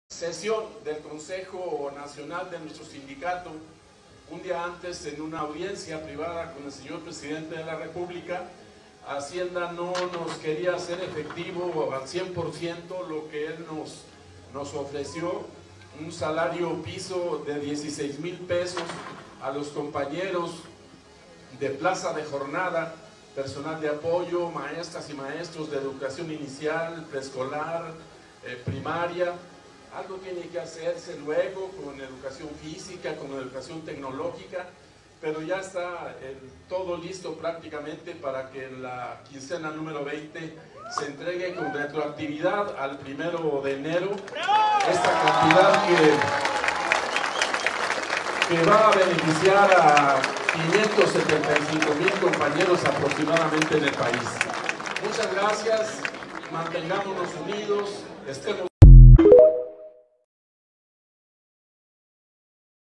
Alfonso Cepeda Salas
Anuncia dirigente nacional del SNTE el pago de nivelación a 16 mil pesos mensuales para personal administrativo, de apoyo y educación inicial en la quincena 20.